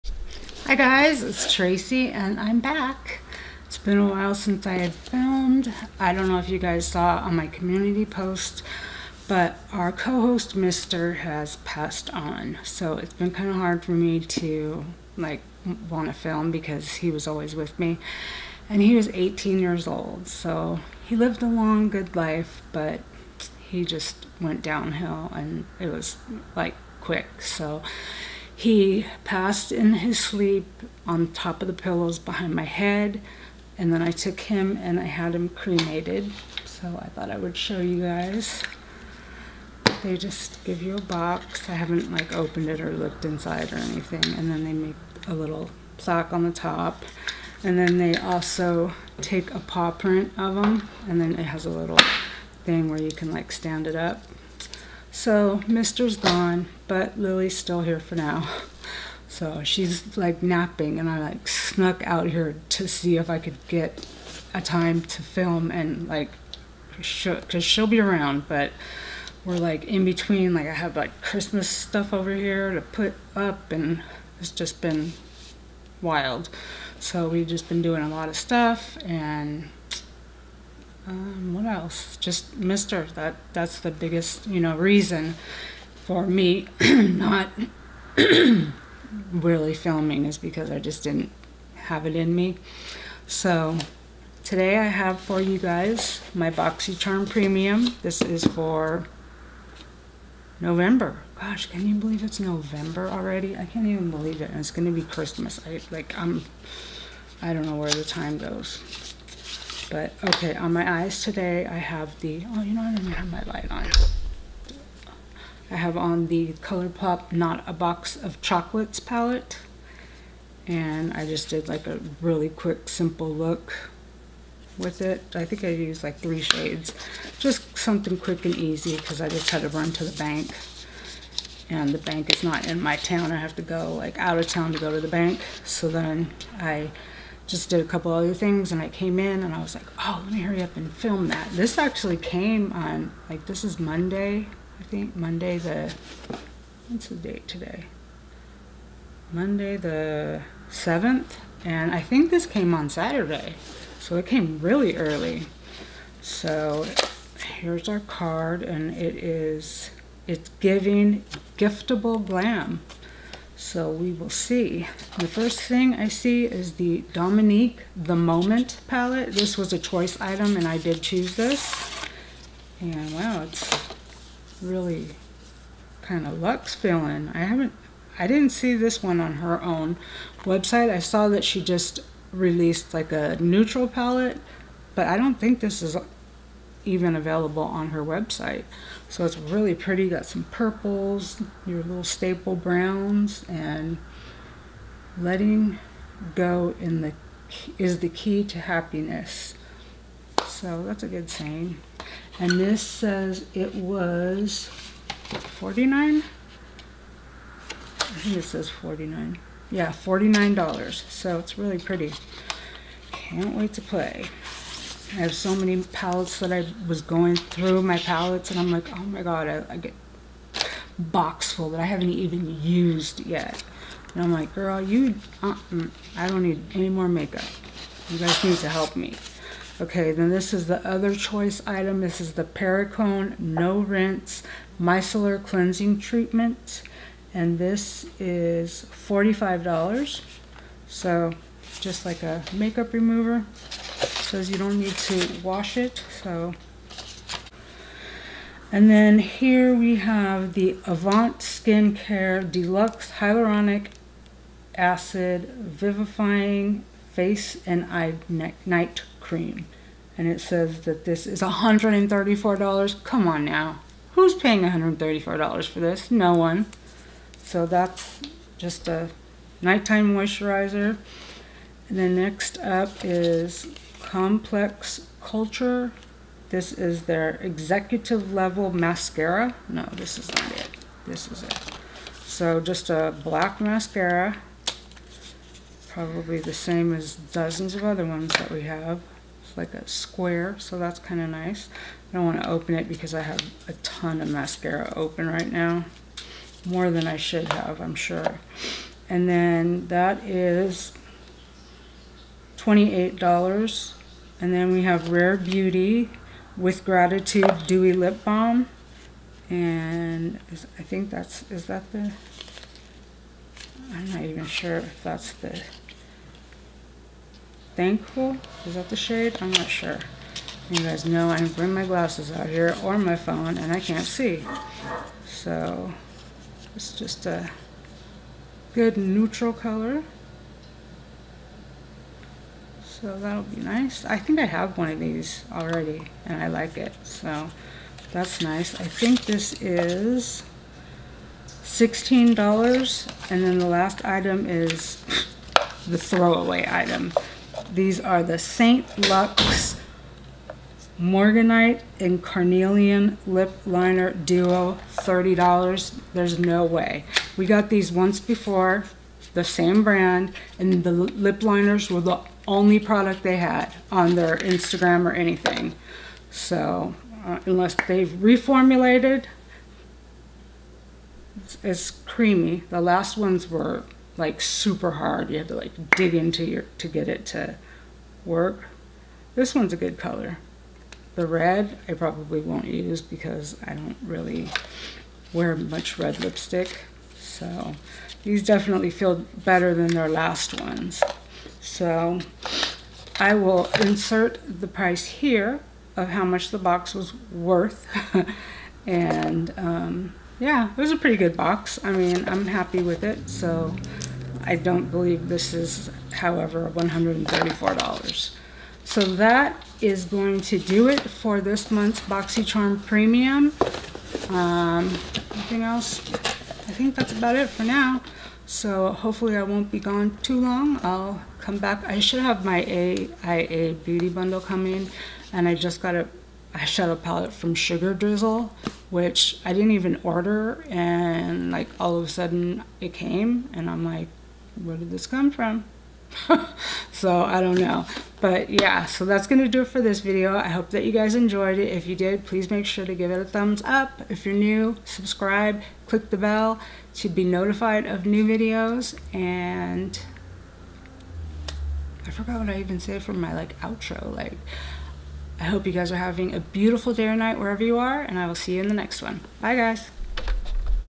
I’m Back!!! November 2022 Boxycharm Premium!!! It Got Here Early!!!😃
Hi Guys!
I’m back with a Boxycharm Premium for November 2022!
I hope to be bringing you regular content, things have just been on a low and hopefully we will be on an up now!

On my eyes: Colourpop Nor A Box Of Cocolates
Lips: Lip Balm